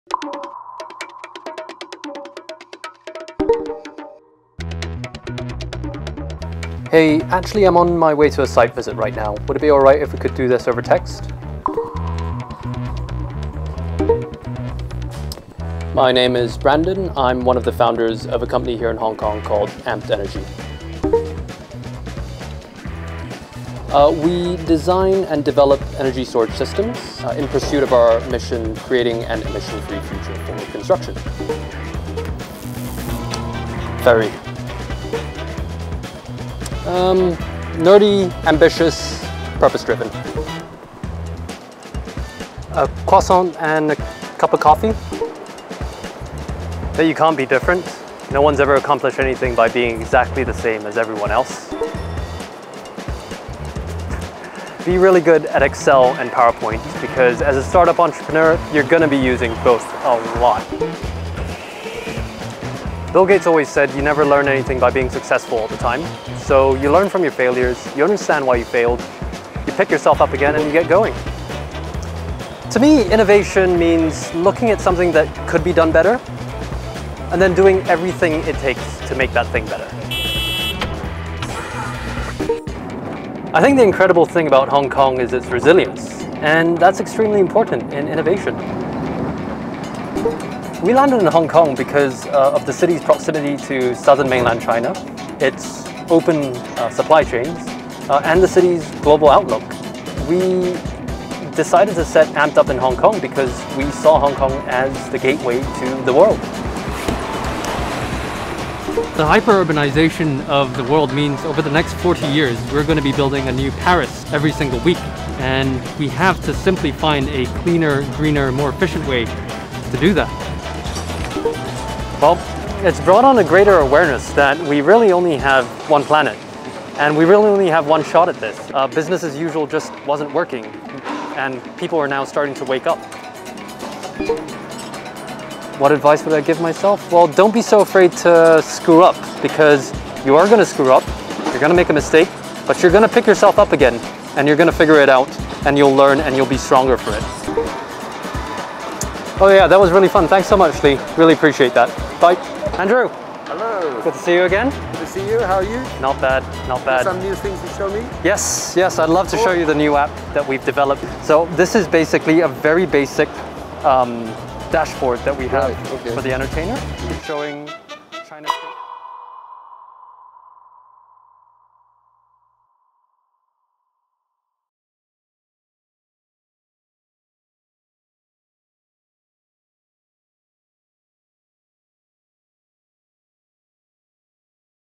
HKSTP x GenT | This Hong Kong Entrepreneur Is Making Construction More Sustainable. Here’s How
A Greener Future Starts Here

“The hyper-urbanisation of humanity over the next 40 years means the world will be building the equivalent of a new Paris every week—and we need to find a greener way to do that,” says Brandon Ng, co-founder and CEO of Ampd Energy, the Hong Kong-based construction tech startup.

Read the full story to uncover the inner spark of Brandon, GenT 2021 Leader of Tomorrow, why sustainability matters to him (and the rest of the world)!